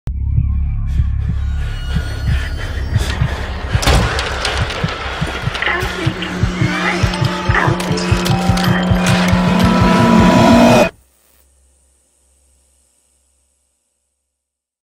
Possessed Pictures (2010s)
Once found on Aaron Becker’s Vimeo page, and disappeared shortly after, so this logo is unused.

Only low resolution is available. There is no higher quality version found whatsoever.